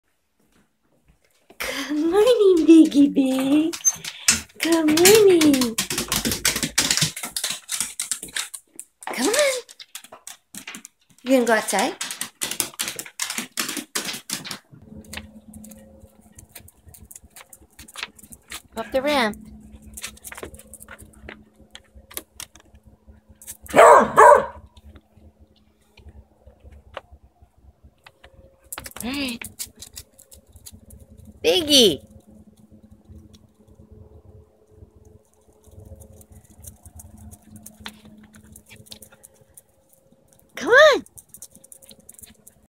GEEZERCAM! Biggie Corgipants' Morning Gallop
Almost 14-year-old Corgi, Biggie wakes up frisky, ready to start a new day. He makes us laugh with his gallop. You can follow his and Benny's adventures on Facebook at Benny Corgipants.